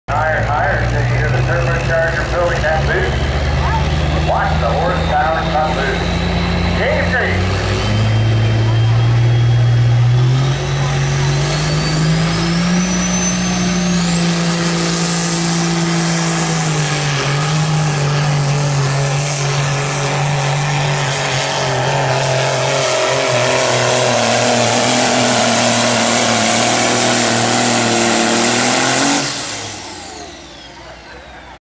Dave Greek "Pressure TIme" limited pro semi pull at the buck
Truckers day at the Buck 8/18/18